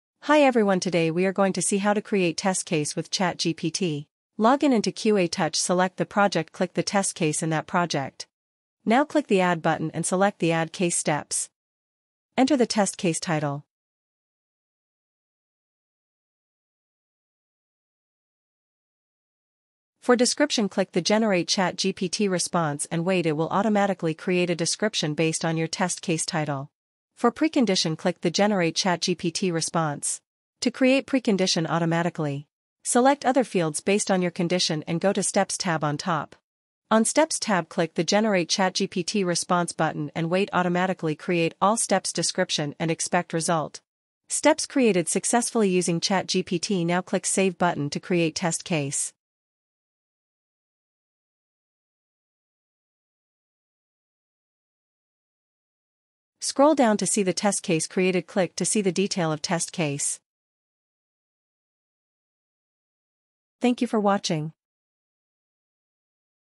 Create Test Cases in QA Touch Faster with ChatGPT
You can write test cases faster in the test management tool QA Touch powered by ChatGPT.
With the ability to generate test cases rapidly with ChatGPT users can significantly reduce the time and effort required for test case creation. This is particularly beneficial in projects with tight deadlines.